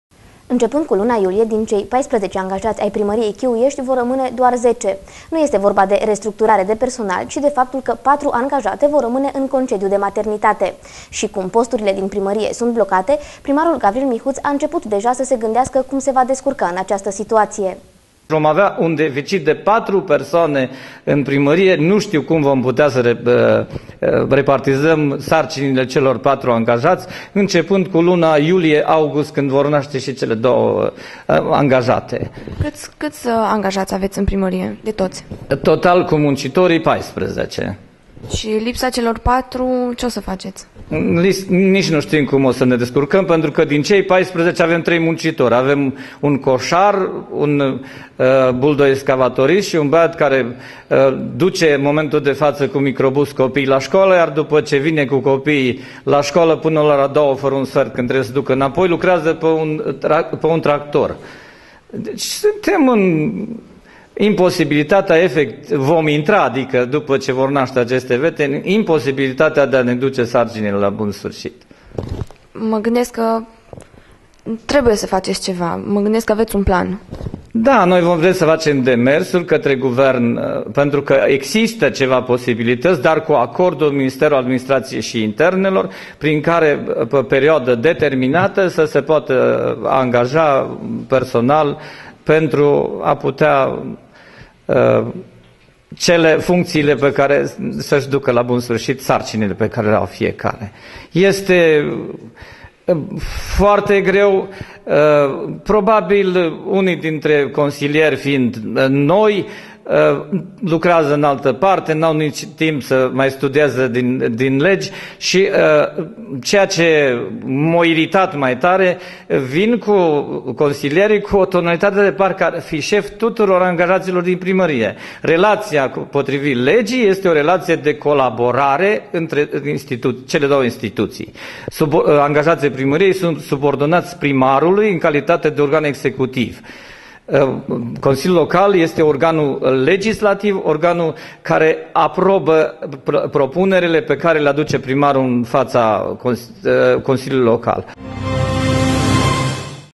07_Personal Mai Putin In Primaria Chiuiesti
interviu cu primar Vasile Mihut